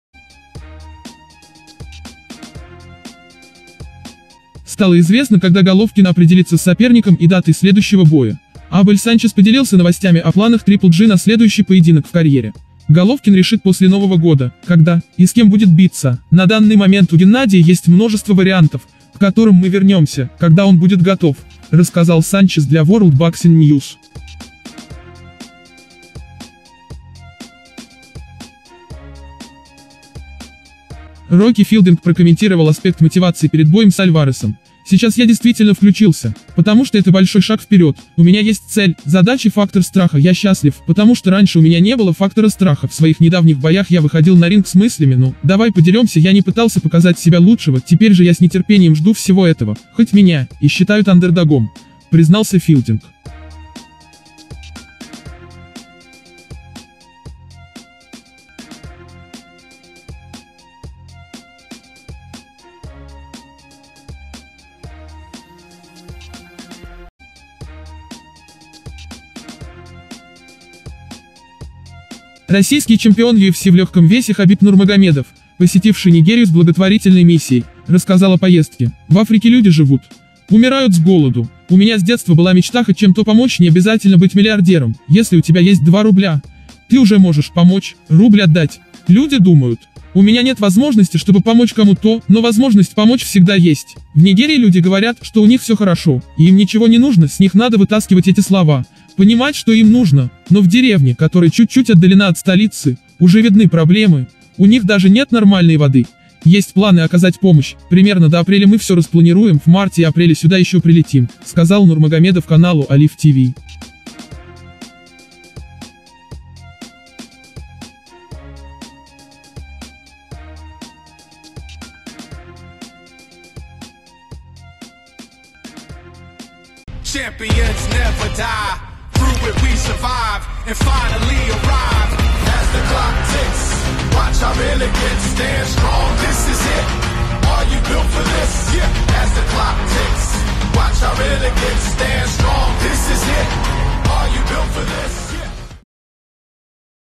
Стало известно дата боя Головкина/ Филдинг боится Канело/ Нурмагомедов – о поездке в Африку.
Новости Бокса и Футбола. новости…

►Канал «FOOTBOX» - самые важные и новые новости Бокса и Футбола.

►В этом выпуске следующие новости:

✔Стало известно, когда Головкин определится с соперником и датой следующего боя.

✔Рокки Филдинг боится «Канело» после его победы над Головкиным.

✔Нурмагомедов – о поездке в Африку: каждый способен помочь нуждающимся.